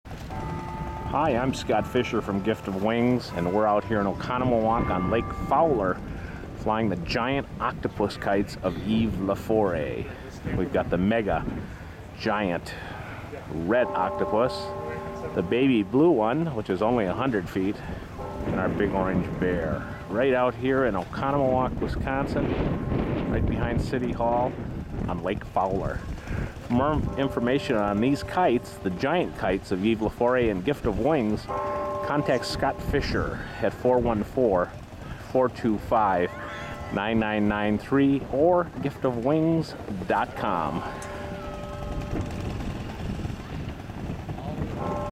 Gift of Wings is flying the Giant Kites of Yves Laforest over Lake Fowler in Oconomowoc, WI
Dan Jones from Gift of Wings is flying the Giant Kites of Yves Laforest over Lake Fowler in Oconomowoc, WI.  Thanks to Brett, and Mark for their assistance.